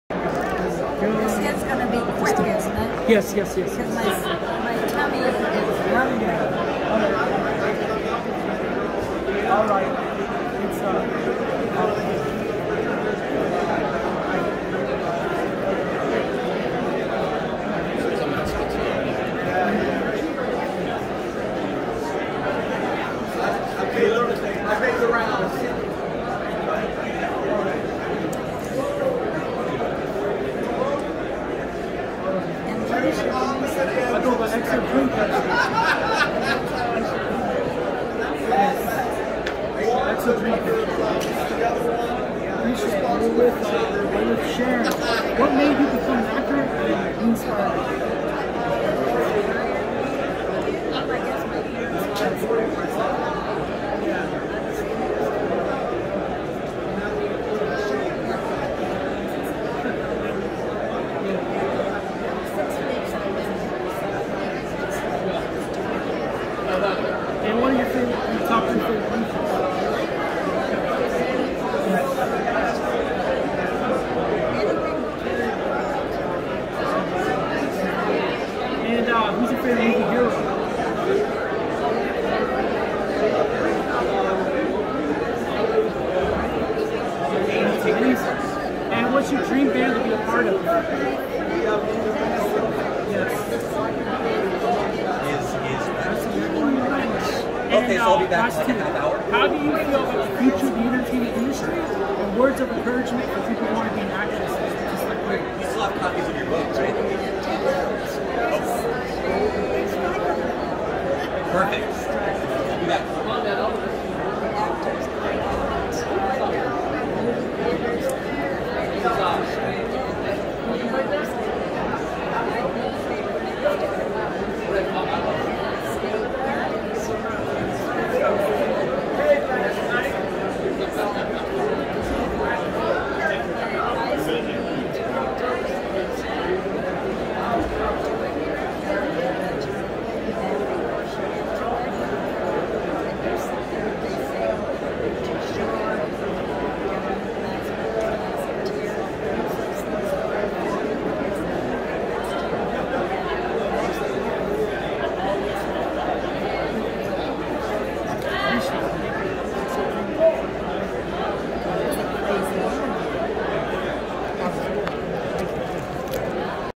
TheXLTE part 2 interviews Sharon Baird unedited version July 1st 2023
Hollywood Show at Marriott Hotel in Burbank California